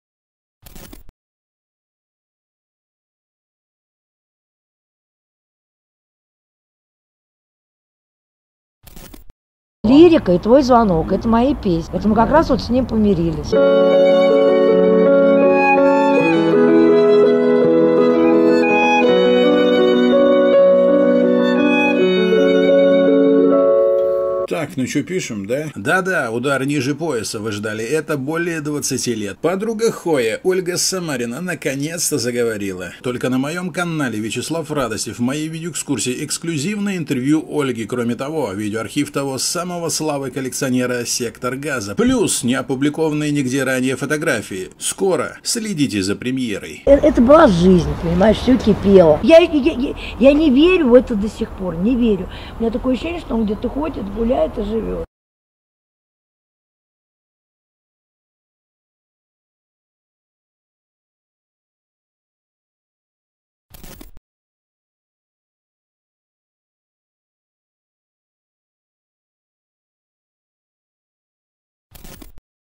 Тизер: Интервью с Ольгой Самариной, Видео-архив Хоя.
Тизер, Интервью с Ольгой Самариной
Юрий Николаевич Клинских ( ХОЙ )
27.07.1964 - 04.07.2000
Основатель и лидер воронежской рок- группы
"СЕКТОР ГАЗА"
4 июля 2000 года от нас ушел человек легенда,- музыкант, вокалист, и автор песен
Юрий Клинских.
Его проект "Сектор газа" за годы существования стал поистине университетом миллионов, на котором выросло не одно поколение. Каждый новый альбом "Сектор газа"
- это необыкновенный коммерческий и музыкальный успех, это первые строчки чартов
продаж по всей стране, миллионные тиражи пиратских кассет , дисков и народная любовь,
которую не купить ни за какие деньги.
Гаишник, грузчик, оператор станков с ЧПУ - он
всегда был со своим народом и видел эту жизнь
без розовых соплей. Он не искал дешевой популярности и никогда не стремился к тому
чтобы его узнавали в лицо.
Юра ушел тихо, не дожив всего несколько дней
до своего 36-ти летия. Судьбе наверное было
угодно чтоб Юра запомнился нам молодым,
тем самым бесшабашным "Колхозным панком"
которым его полюбила вся страна.